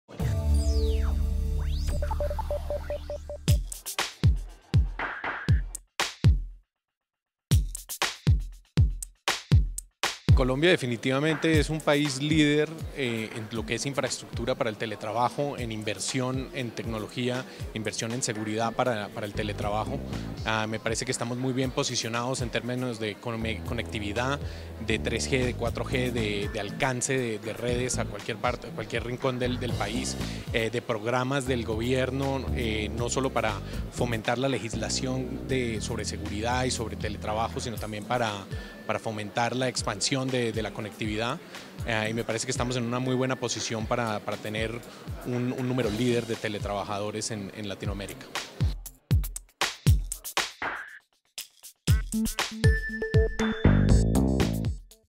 Ernesto Kling toma a Colombia como referente latinoamericano en Teletrabajo
Ernesto Kling, especialista en movilidad de Citrix Systems para Latinoamérica, ve a Colombia con muchos avances en temas de conectividad, razón por la cual el país viene en constante aumento con la cultura del Teletrabajo.